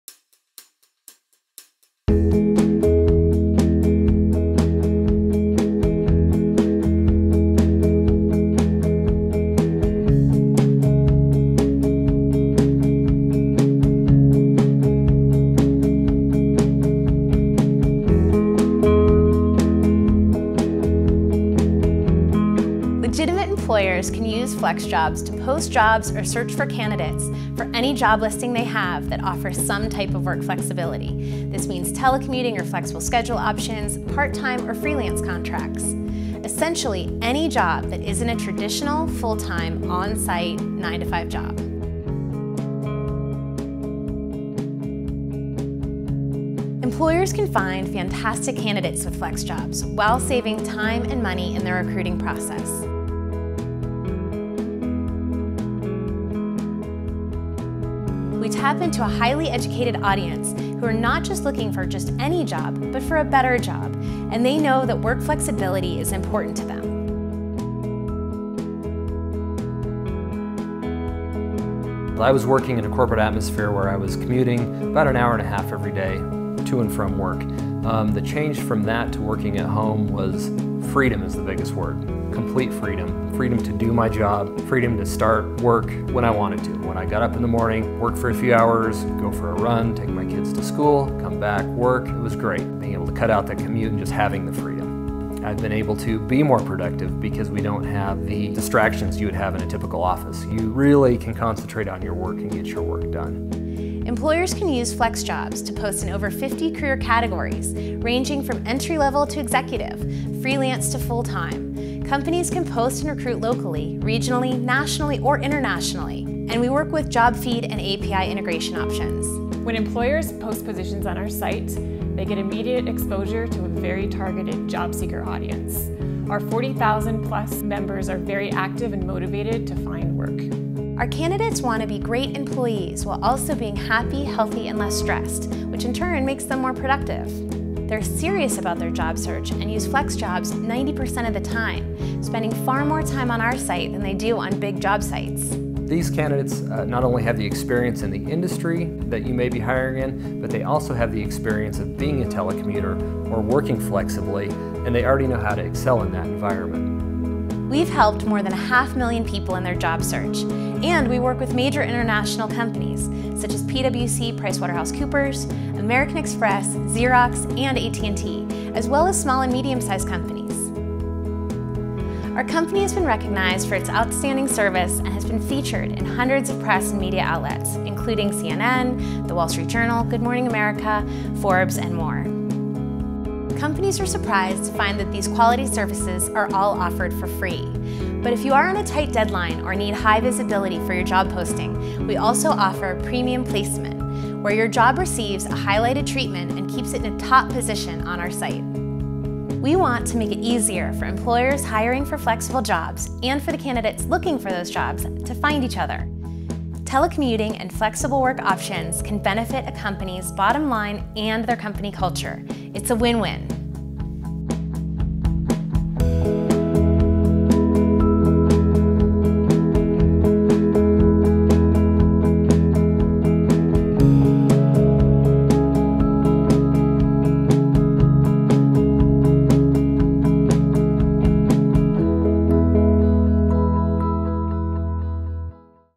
How to Hire Remote, Freelance, Part-time, and other Flexible Workers
FlexJobs is the leading job site to help employers find quality candidates looking for jobs that offer flexible work options, such